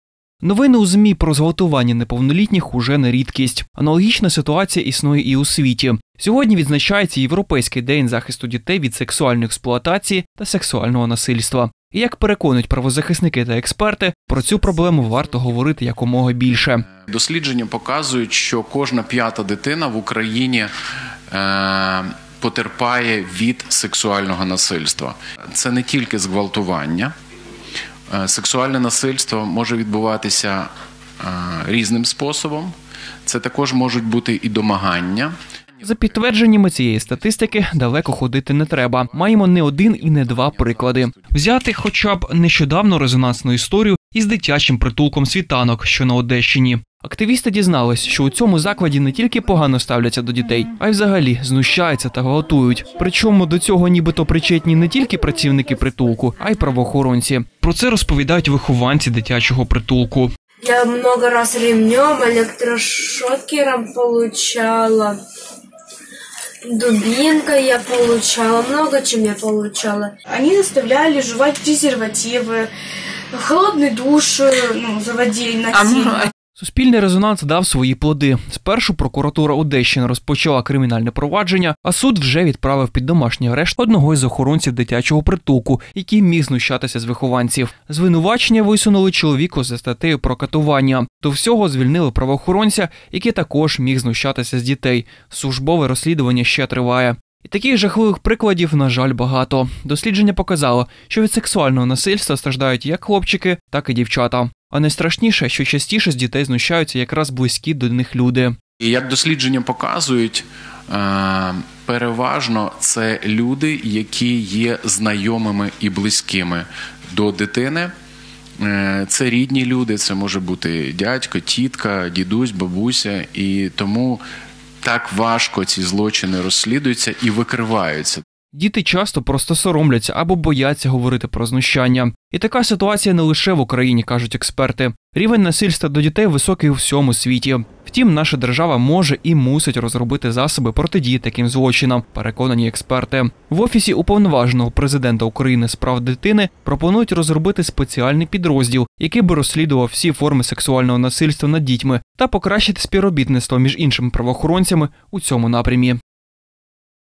Кожна п’ята дитина потерпає від сексуального насильства — Кулеба
Сьогодні  відзначається Європейський день захисту дітей від сексуальної експлуатації та сексуального насильства. І як переконують правозахисники та експерти, про цю проблему варто говорити якомога більше.